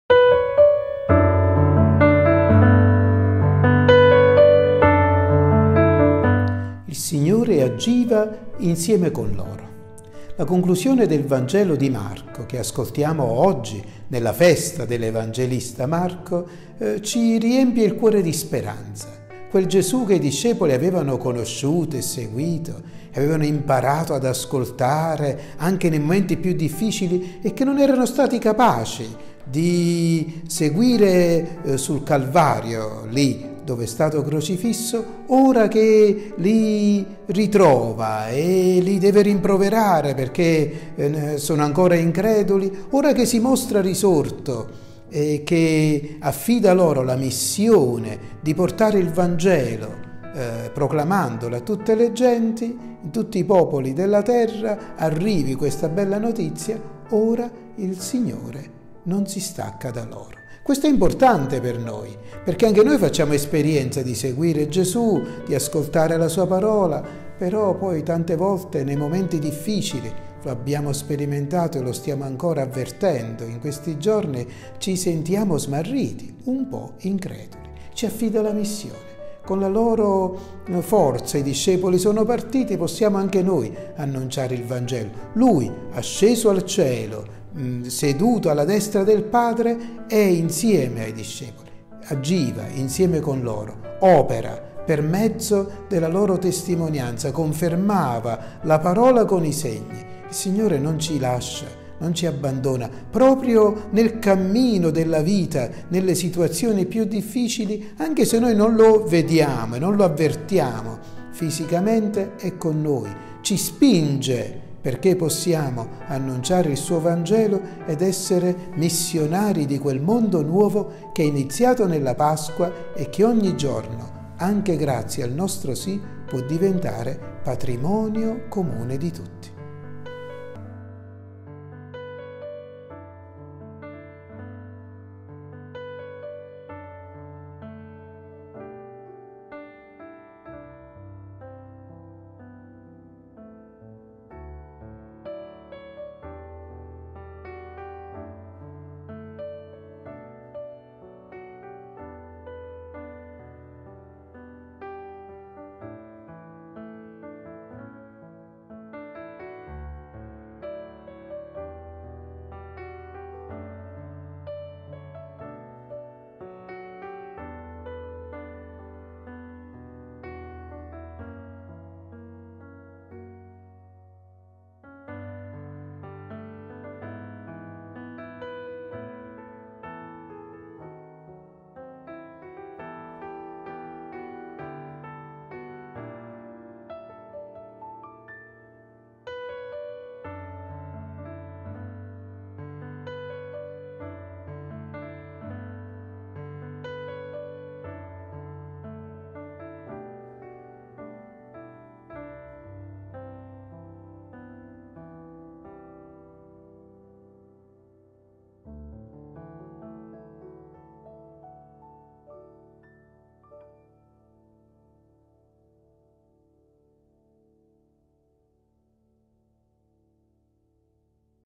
La Parola per ogni giorno 25 aprile 2020
La Parola di oggi è: Il Signore agiva insieme con loro

Lascia un commento, una preghiera, così che insieme possiamo sentirci tutti uniti nel Padre celeste.